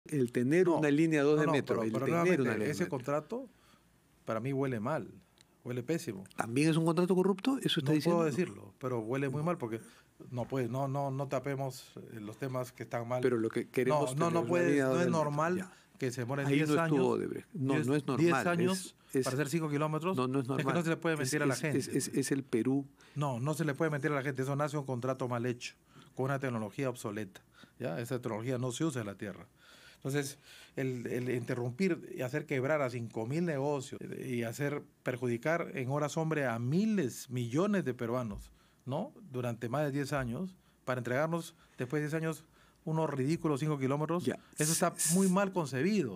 Rafael López Aliaga sobre Línea 2 del Metro: “Ese contrato huele mal, está mal hecho”
🔴 En una entrevista exclusiva, el alcalde de Lima, Rafael López Aliaga, consideró que el contrato firmado entre el MTC y el consorcio encargado del proyecto Línea 2 del Metro de Lima “está mal hecho y huele mal”.

📱 Más información en rpp.pe